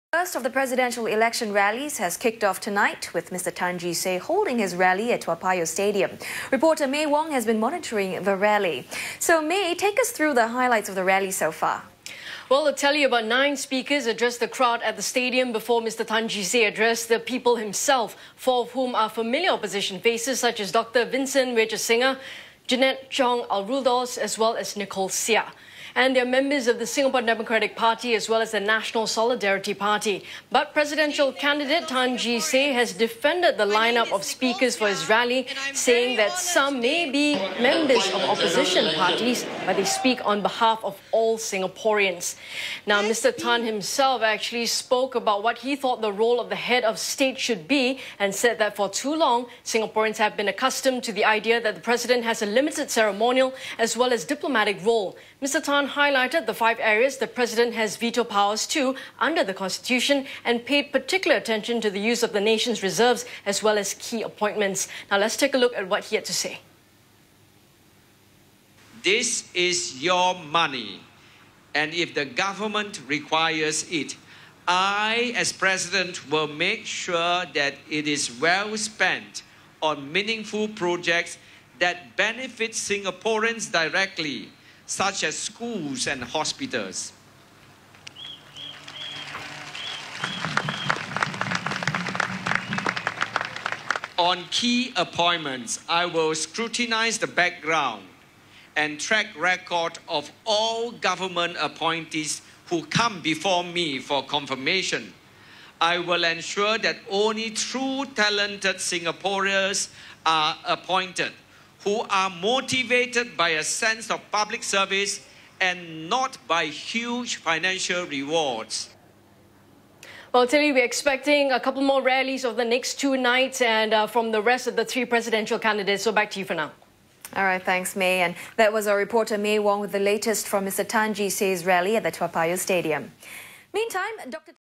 PE: Tan Jee Say holds first rally in presidential race - 23Aug2011
SINGAPORE: Mr Tan Jee Say was the first presidential candidate to hold a rally.

About nine speakers addressed the crowd at the Toa Payoh Stadium before Mr Tan took to the stage on Tuesday.

They included some opposition candidates who stood in the last General Election such as Dr Vincent Wijeysingha, Dr Ang Yong Guan, Jeannette Chong-Aruldoss and Nicole Seah.

Mr Tan defended the line-up of speakers for his rally, saying some may be members of opposition parties but they were speaking on behalf of all Singaporeans.

Mr Tan spoke on what he thought the role of the President should be and said that for too long, Singaporeans have been accustomed to the idea that the President has a limited ceremonial and diplomatic role.

Mr Tan highlighted the five areas the President has veto powers under the Constitution. He paid particular attention to the use of the nation's reserves and key appointments.

He said: "This is your money and if the government requires it I as President will make sure that it is well spent on meaningful projects that benefit Singaporeans directly such as schools and hospitals.

"On key appointments, I will scrutinise the background and track record of all government appointees who come before me for confirmation.

"I will ensure that only true talented Singaporeans are appointed (and) who are motivated by a sense of public service and not by huge financial rewards."

He added that he will examine more closely appointments to government bodies, statutory boards and government-linked companies because "too few people are holding too many portfolios within the government."

If elected, Mr Tan said he would also publish an annual report detailing the custodial, ceremonial and social responsibilities performed by the President during the year.

He noted that Singaporeans are "thirsting for change" and that he intends to "ask the government to do more".

Mr Tan said: "This government needs to be challenged and checked, if we do not want it to become complacent.

"First, I intend to change the direction of the Presidency to move it much closer to the aspiration of the people.

"To do that, I cannot remain within the ivory gates of the Istana. I must move among the people and initiate programmes that galvanise the young and the young at heart and harness their energies to build a better Singapore."

Mr Tan ended the rally with a talkshow format question-and-answer session, where he spoke about his early years and experience in the public and private sectors.